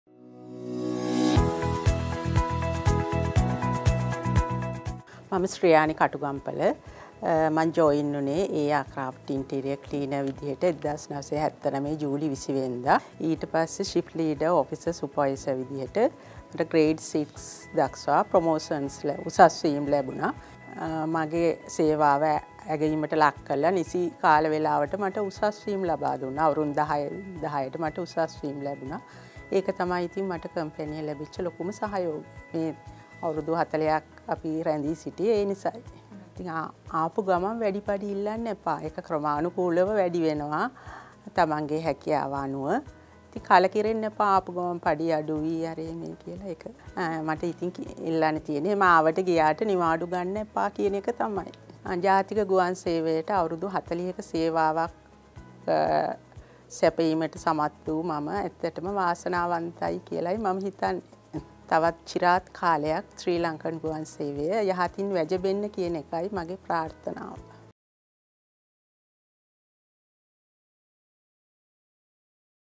Celebrating 40 years of Service
Watch Sriyani Katugampola an Aircraft Interior Service Supervisor share her memories working for the flag Carrier.